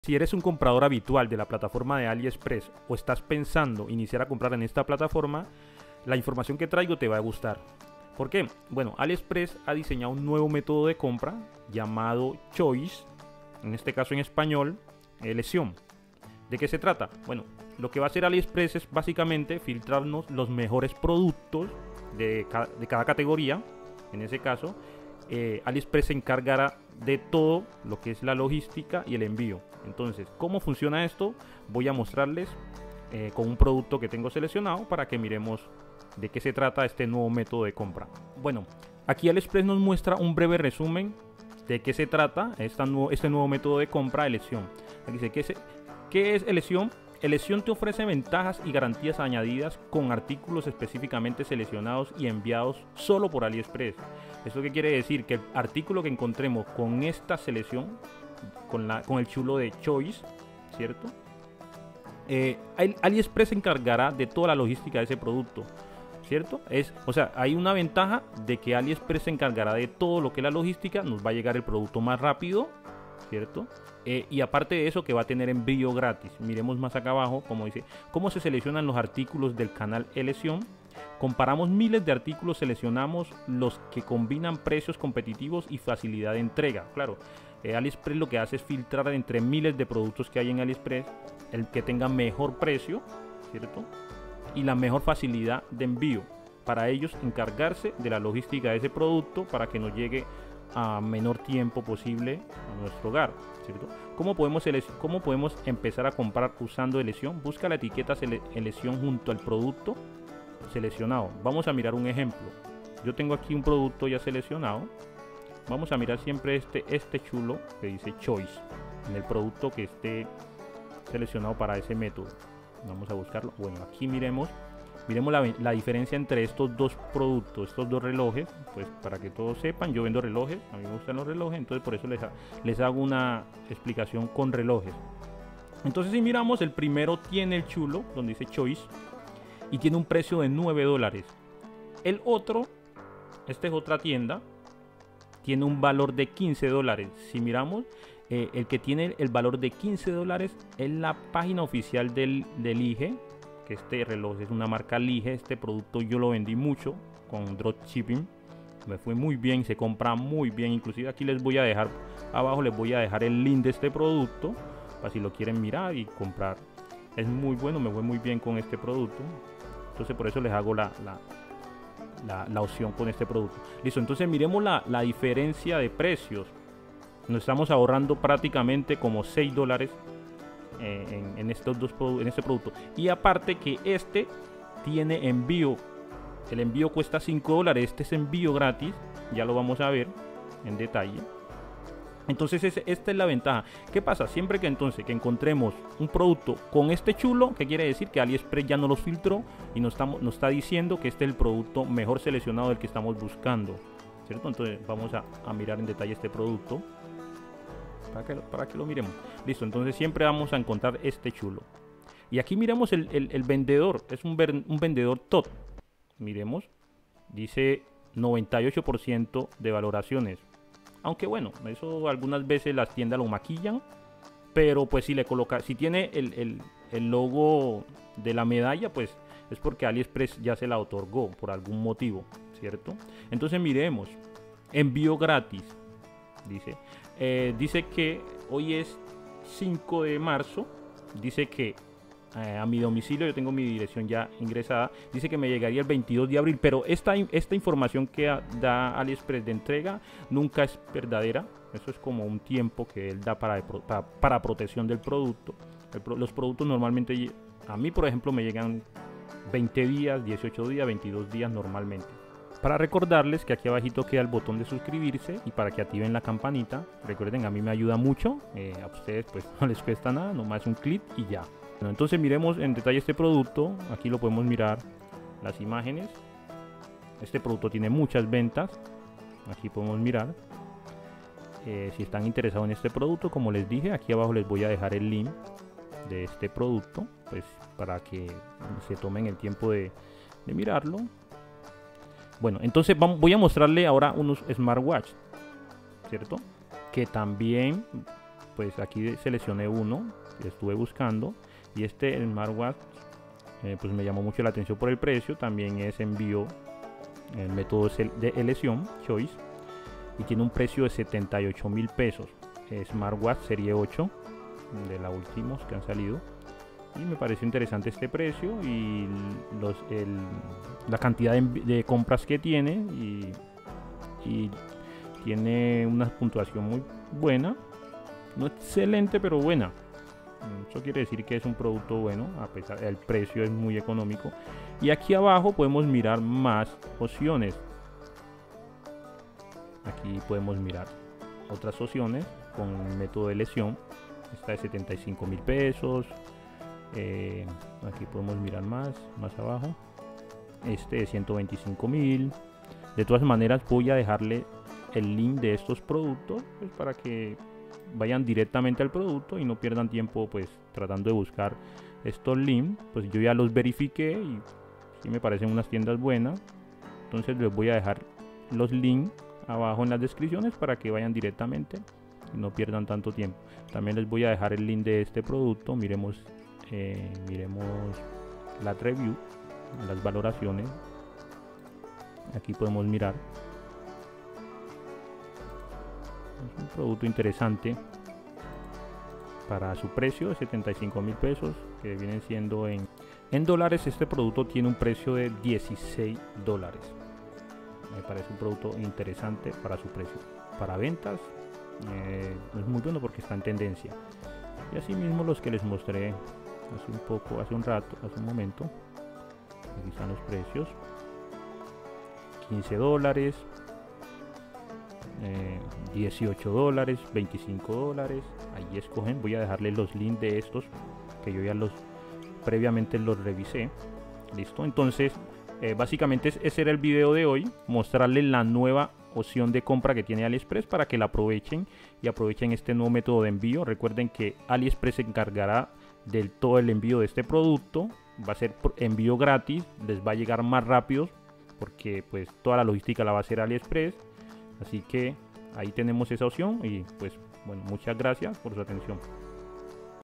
🛍️PRECIOS INCREÍBLES de Aliexpress🎁📦 Con Aliexpress Choice
En este video, te contamos cómo puedes encontrar precios increíbles de Aliexpress con Aliexpress Choice. Esta es una buena manera de ahorrar en tu compra online.Esta página da a los usuarios la oportunidad de hallar precios increíbles en productos chinos, así como en productos moda, electrónica, artículos de viaje, gastronomía, entre otros. En esta página, también se ofrecen ofertas exclusivas y ofertas especiales

Reloj LIge

SmartWatch 8 Ultra  opcion 1

SmartWatch 8 Ultra  opcion 2

SmartWatch 8 Ultra  opcion 3